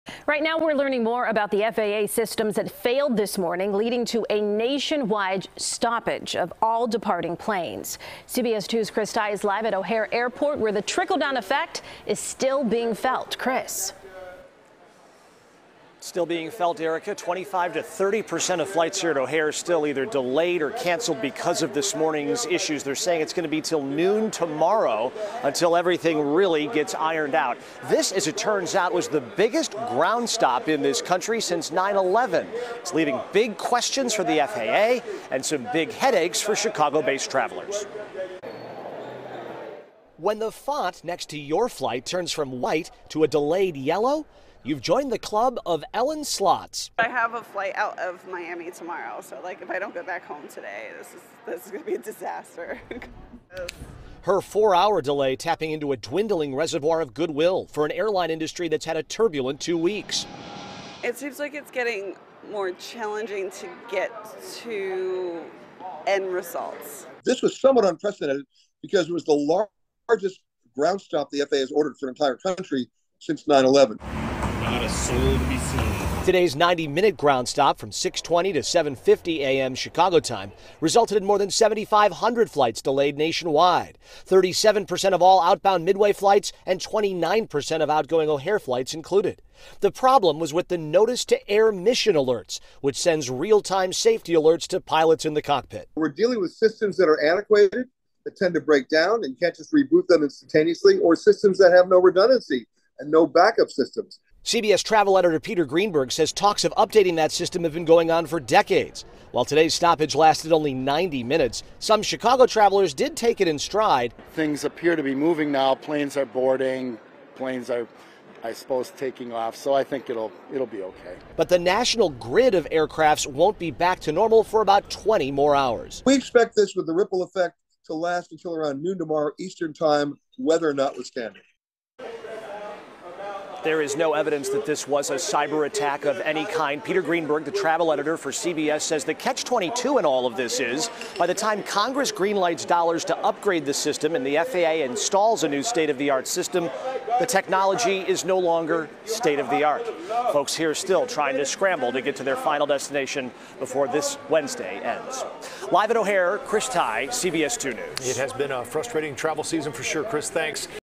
'Somewhat unprecedented' flight stoppage another hurdle for Chicago flyers
Wednesday's failure of the Federal Aviation Administration systems that lead to a nationwide stoppage of all departing planes was unlike anything the country had seen in more than two decades, according to experts.